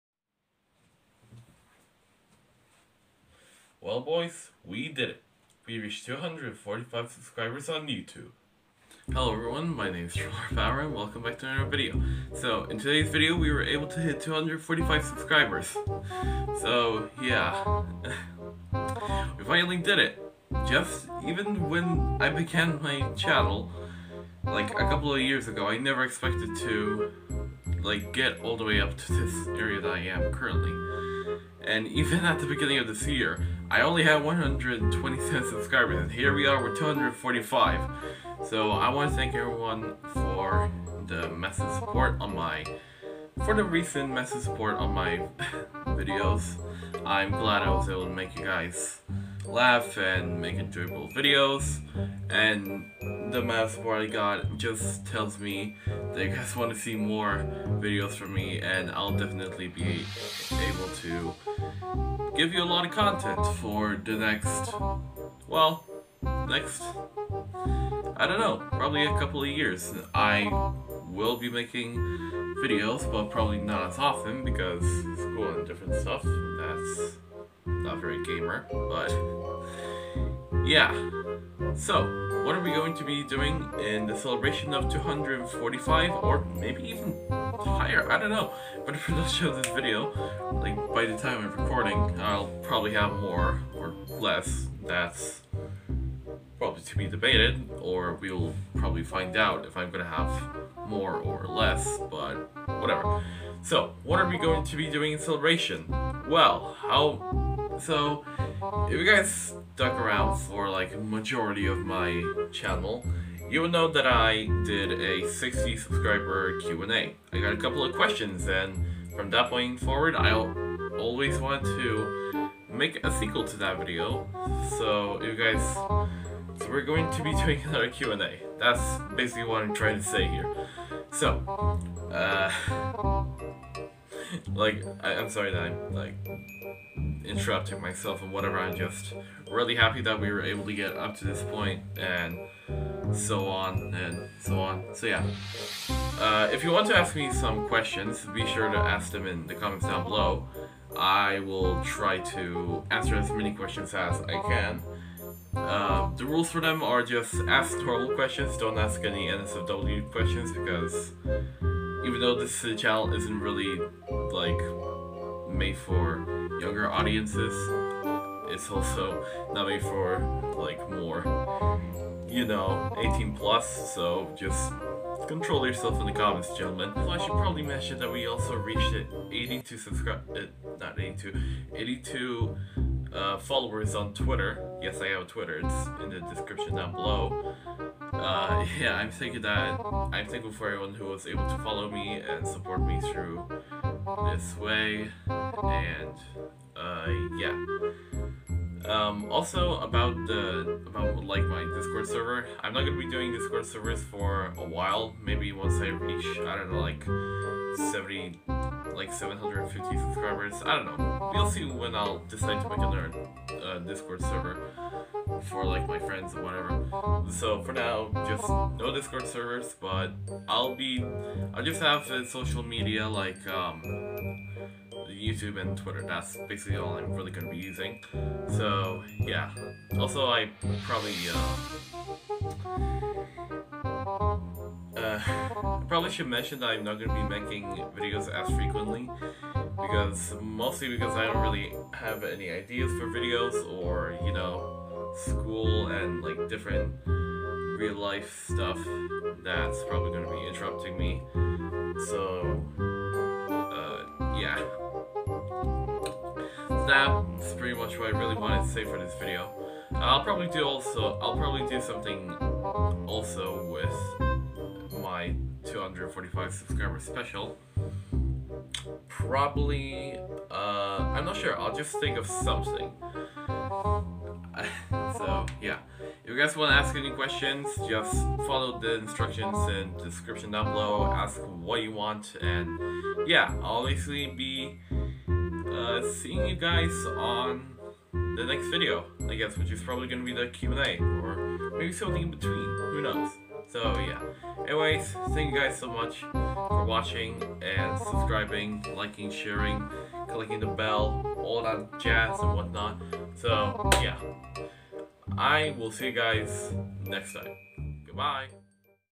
245 Subscriber Q&A Annoucement!
Rules for the Q&A

1.No NSFW questions
2.you are allowed to ask as many questions as you desire
3.dont ask any personal questions like where i live and so on